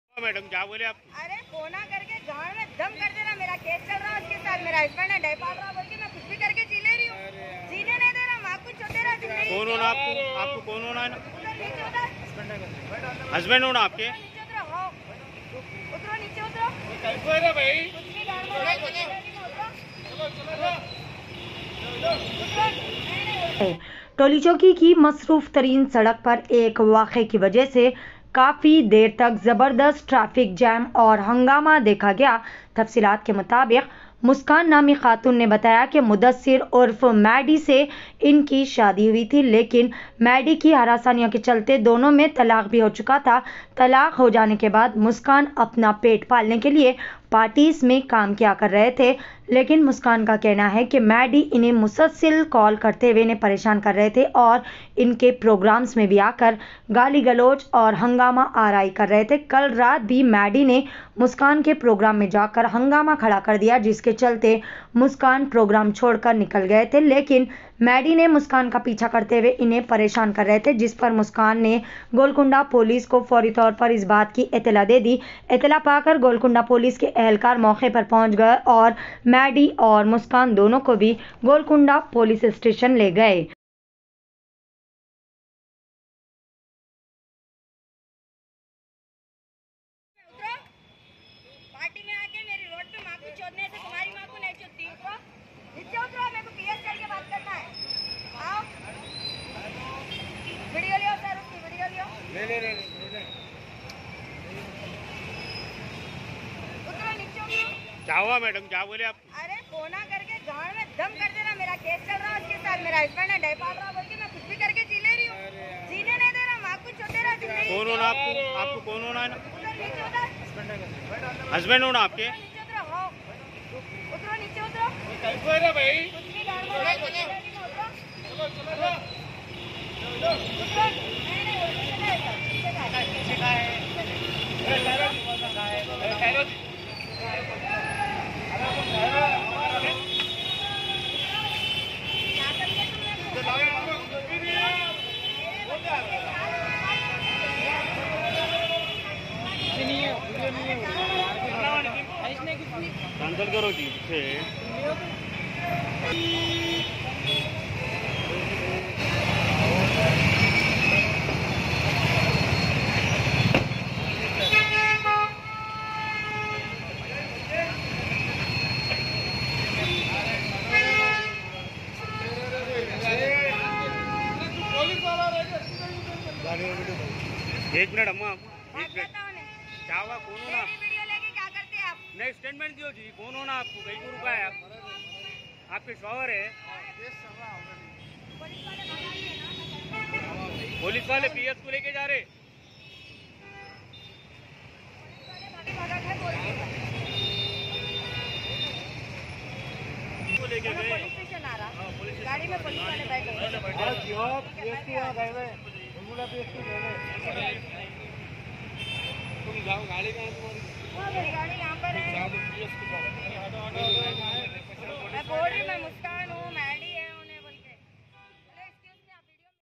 TOLICHOWKI ROAD PAR EX HUSBAND KE TORTURE SE BIWI NE KIYA HUNGAMA TOWLICHOWKI HYDERABAD..
TOLICHOWKI ROAD PAR
EX HUSBAND KE TORTURE SE BIWI NE KIYA HUNGAMA TOWLICHOWKI HYDERABAD..